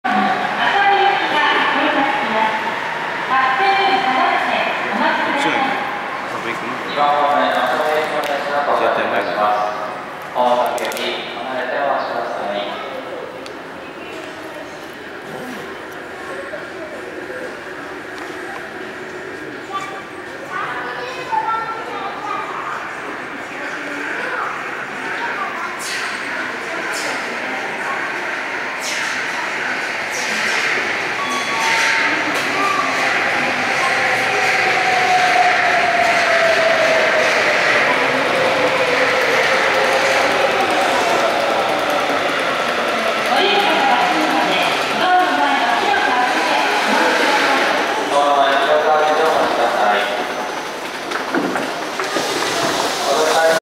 【札幌市営地下鉄】南北線 可動式ホーム工事中のさっぽろ駅
撮影：2012/9/23
さっぽろ駅の可動式ホーム設置完了は27日の予定だそうです。
いずれ貴重な映像になると思う（？）ので撮っておきます。
麻生行きの車両が入ってくる様子です。ホームドア付いてからも「白線より下がって～・・・」が聞けるのはいまのうちでしょうか・・・